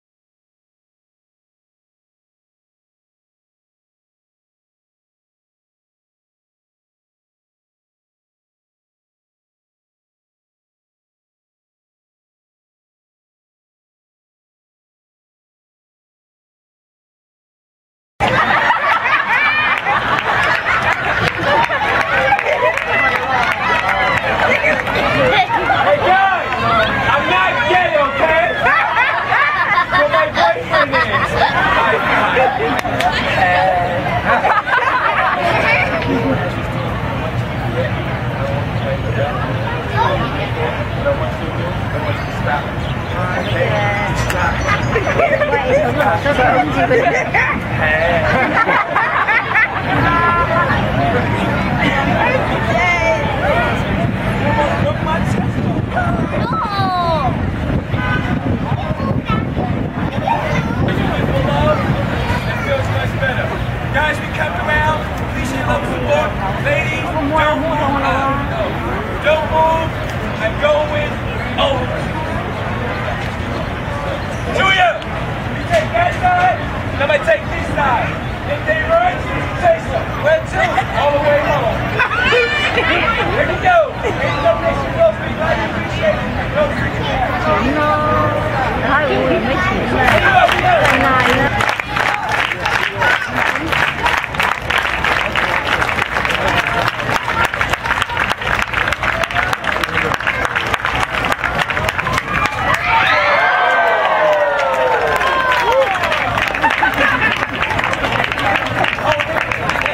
Man Jumps Over Six Women at Union Square Park
New York City, Union Square Park